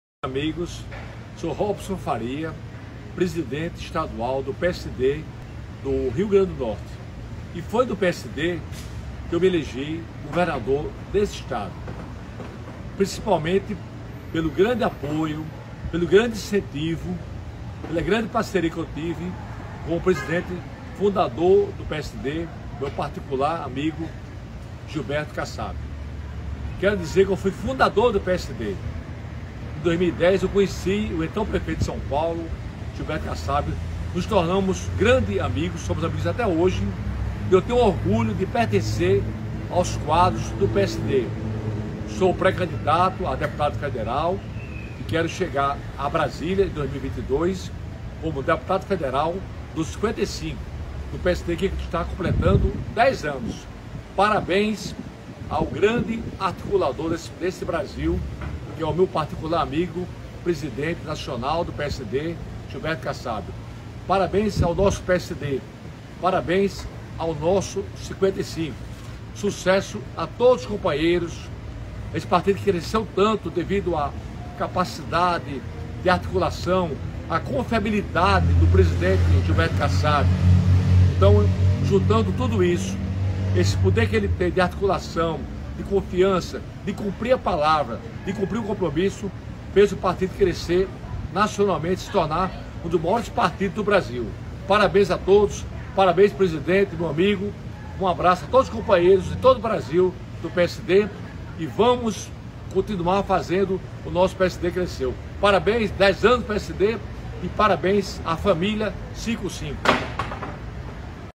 Robinson Faria - PSD 10 anos
Presidente do PSD no Rio Grande do Norte e um dos fundadores do partido, Robinson Faria, deseja sucesso a todos os companheiros de partido.